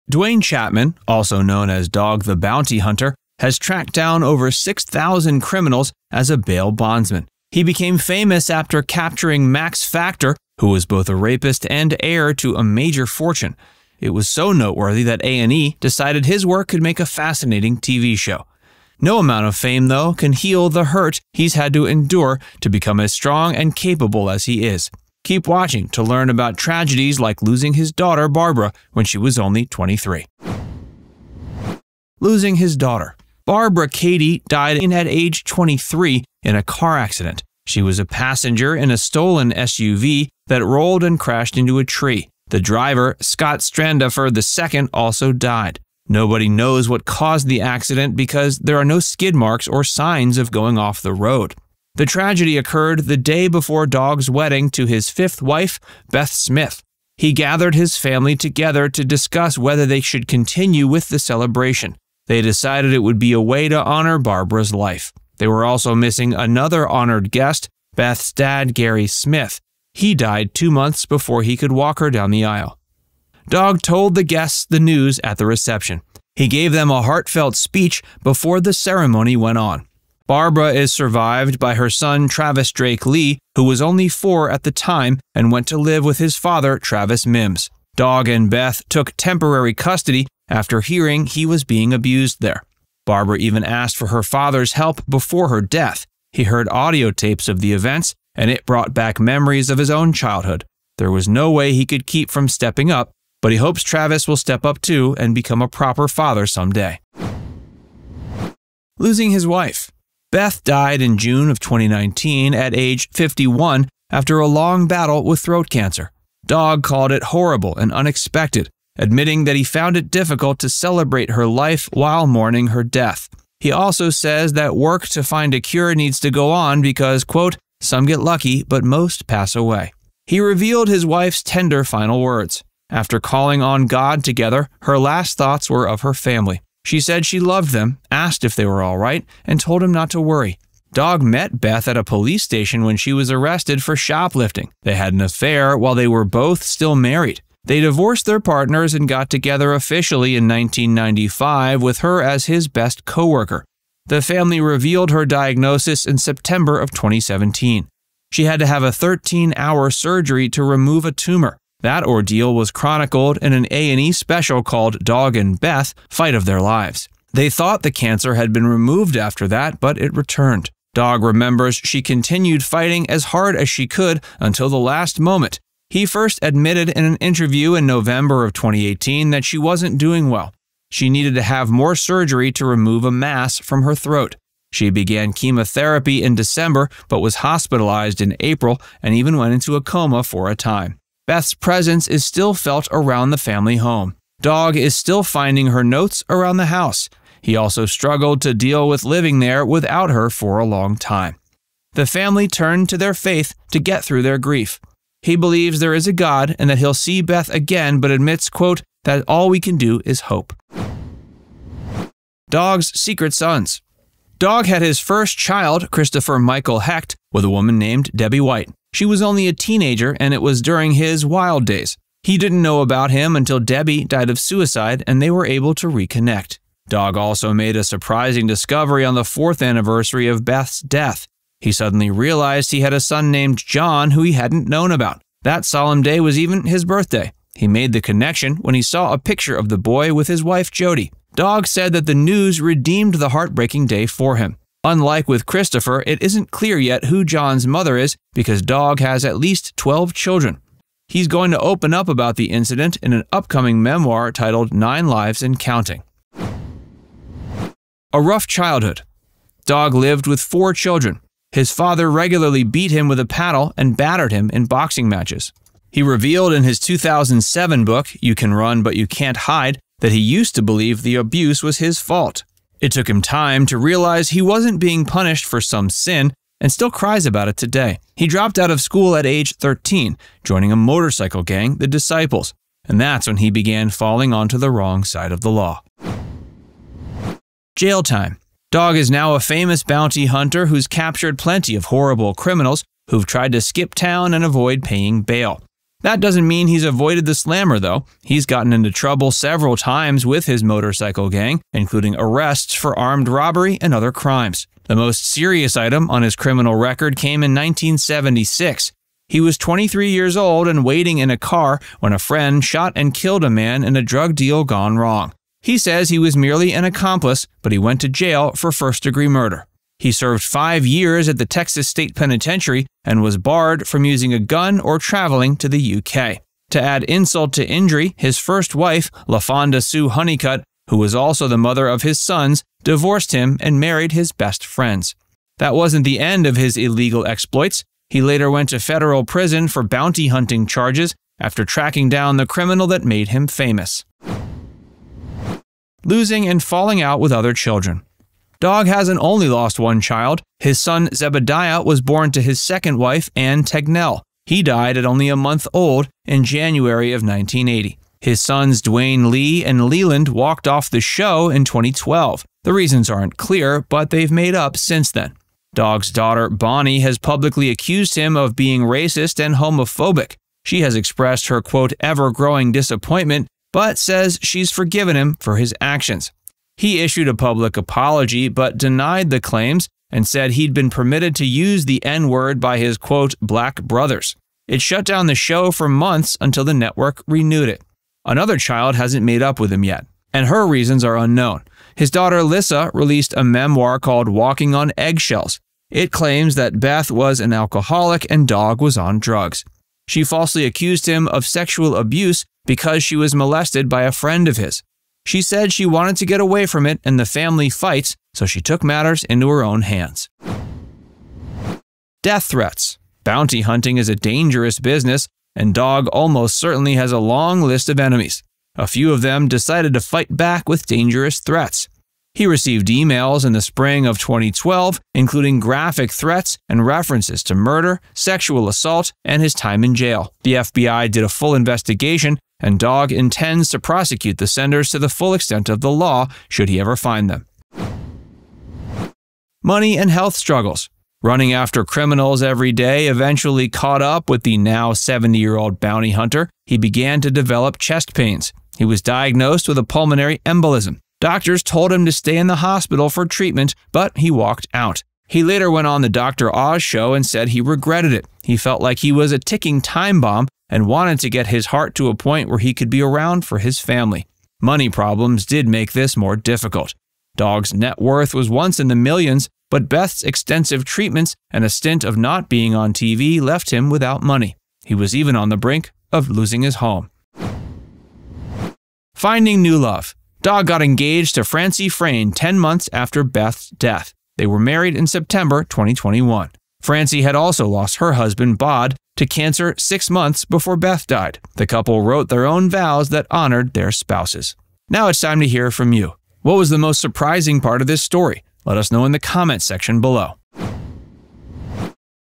Barbara, Dog the Bounty Hunter’s Daughter, Dead at 23
What’s your favorite reality show redemption story? Which shows have stars who made their lives better instead of worse after becoming famous? Dog the Bounty Hunter had nowhere to go but up after his difficult childhood. He had an abusive father and fell in with a biker gang. That led to a murder case where he spent five years in jail. Then, he began tracking down criminals instead of becoming one. He bagged a serial rapist, and it got the eye of TV networks. They decided that it might be entertaining to watch him chase down the bad guys. It worked and gave him global recognition. What it didn’t do was stop him from experiencing tragedy after tragedy.

00:00 - Intro
00:31 - Losing His Daughter
01:48 - Losing His Wife
03:32 - Dog’s Secret Sons
04:18 - A Rough Childhood
04:47 - Jail Time
05:45 - Losing and Falling Out With Other Children
06:51 - Death Threats
07:19 - Money and Health Struggles
07:59 - Finding New Love
08:14 - Outro

There was the loss of his wife Beth. She faced a courageous but difficult battle with throat cancer until she let go and told her family not to worry about her. Dog was a bit of a lady's man before meeting her. He has many children, and their relationships aren’t all great. There were two sons he didn’t know about. He discovered one on the anniversary of Beth’s death. He found out about his first child after the boy’s mother committed suicide.

The children he does know don’t all speak to him. His daughter Bonnie believes he is racist and homophobic. His other daughter Lyssa made a false sexual assault claim against him. She also alleges that living with him was draining because he was addicted to drugs and Beth was an alcoholic. Dog has also lost children. His son Zebedee only lived for a month. That was the first time, but not the last.

Watch our video to learn about the death of Dog the Bounty Hunter’s daughter Barbara.